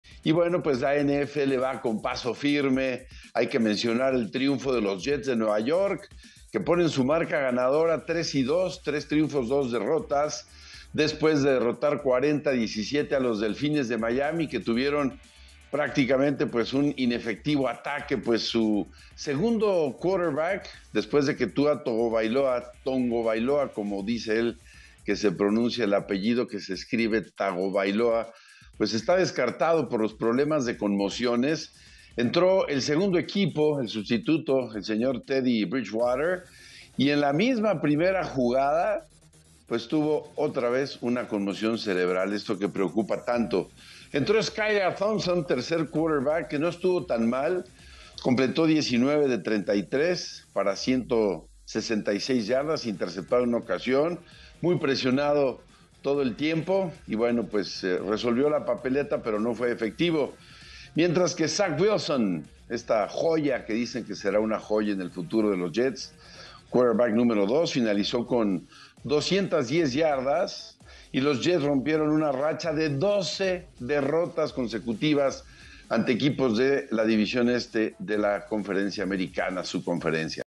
Los Jets de New York aplastan a Miami | Imagen Deportes | Palabra del Deporte
Los Jets derrotan 30 a 17 a los Dolphins y rompen su racha de derrotas.